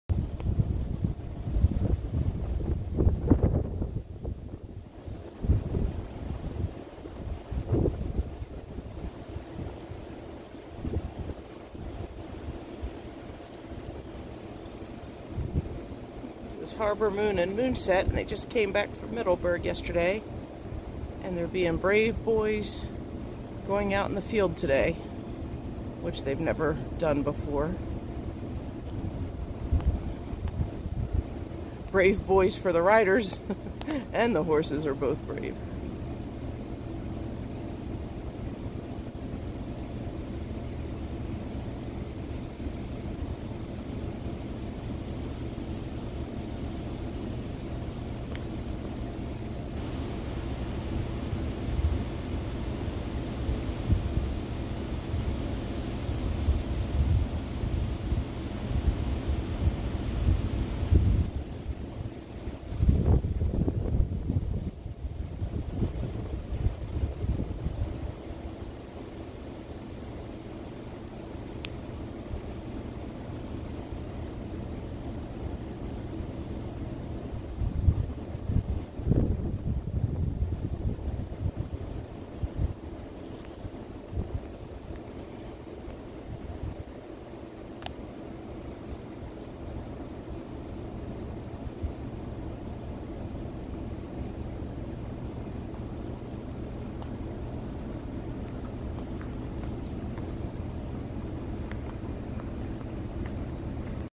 Harbor Moon (light blue cap) and Moonset (royal blue cap) jog in the fields on their first day back.
They returned from Middleburg Training Center where they had been galloping for 6 months while being VA-certified.